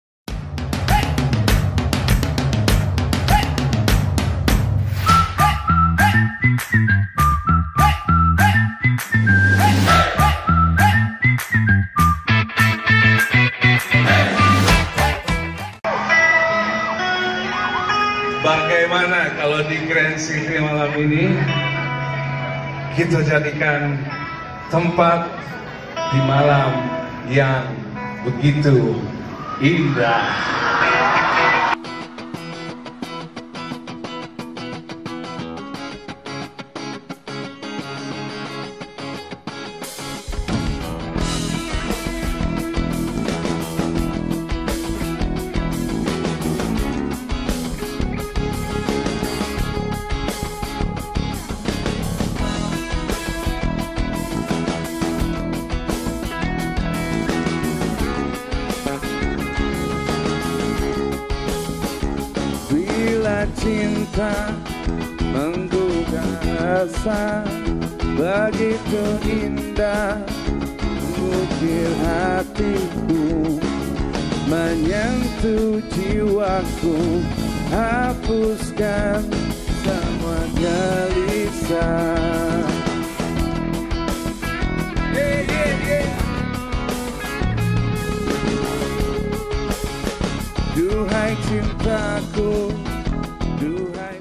Caffino | Jatim Fair 2019 Door Price Kawasaki W175
Kemeriahan Jatim Fair 2019 yang mana tahun ini begitu dahsyat dengan total pengunjung 102.000 orang. Ditambah lagi dimeriahkan Oleh Caffino Kopi dan Susu Asli yang Authentic jadi lebih Nikmati mendengarkan Alunan Lagu yang dibawakan Oleh